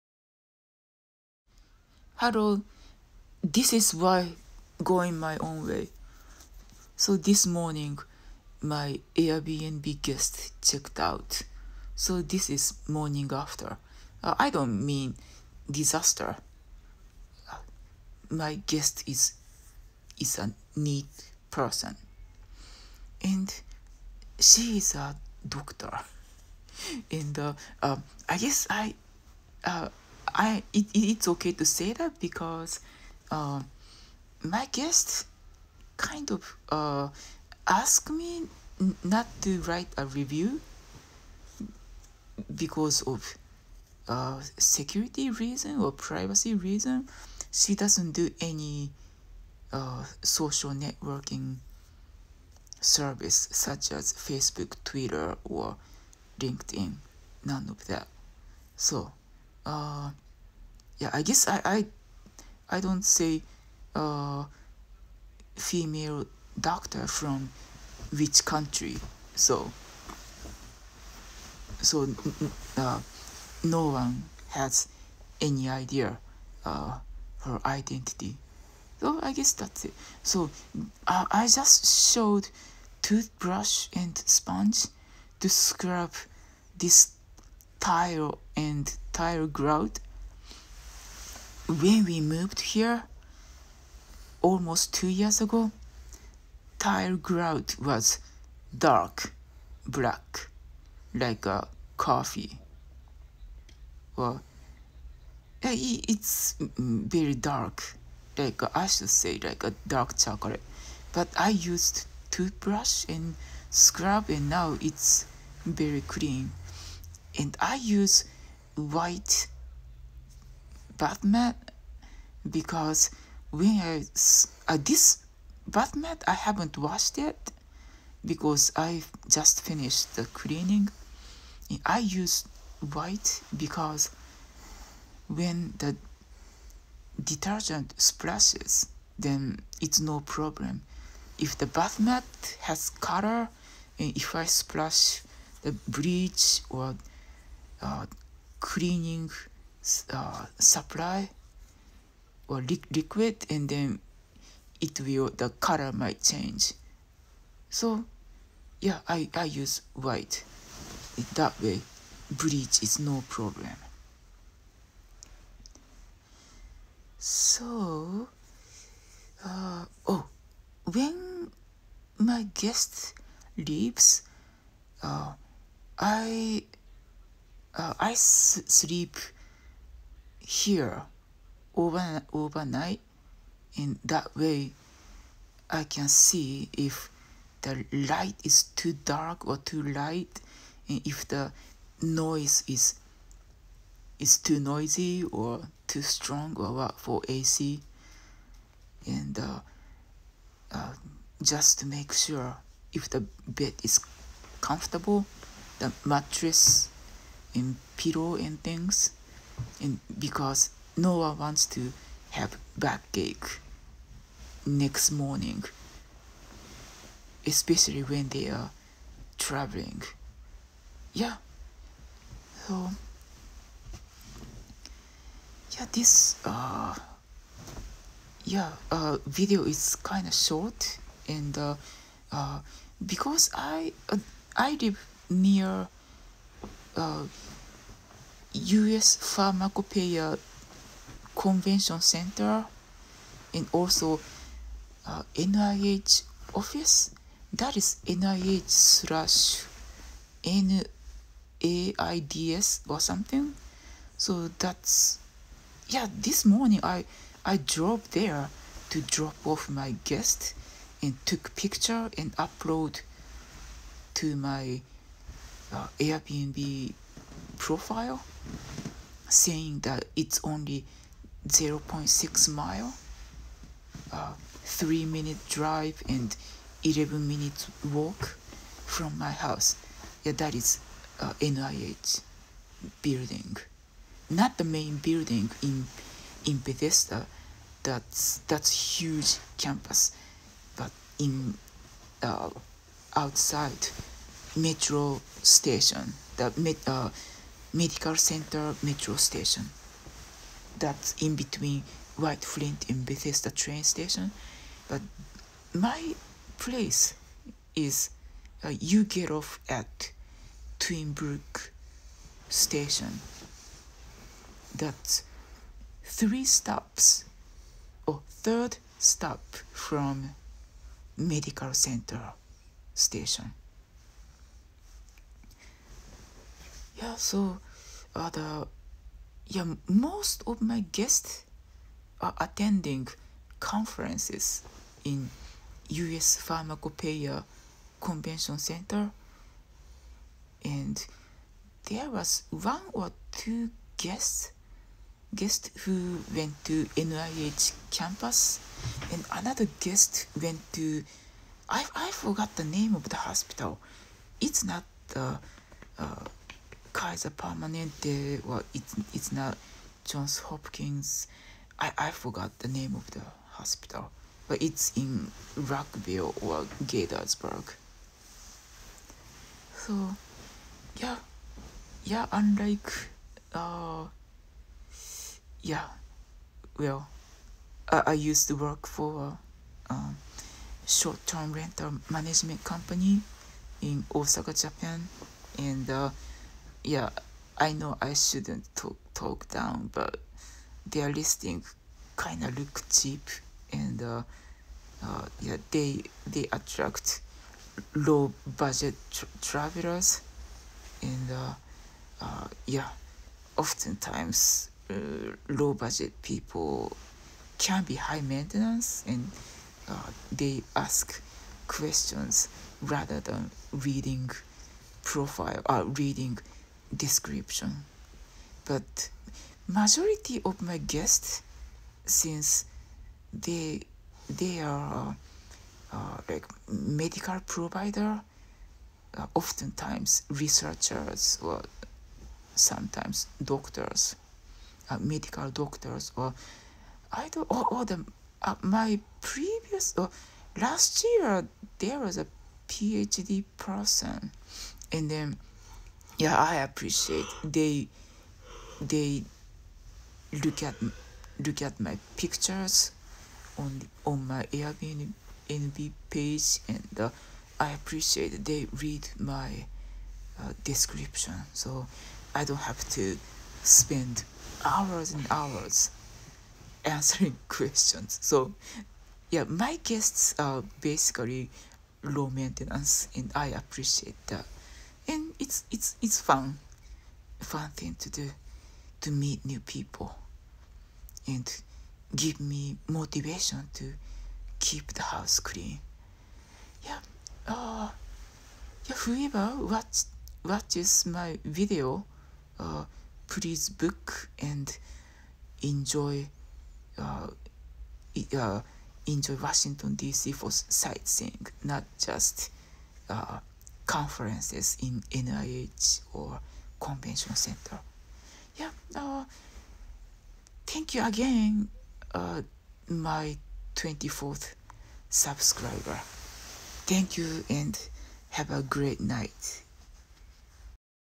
Airbnb guest checked out and its morning after
Cleaning and check the bed if everything is clean and the bed is comfortable.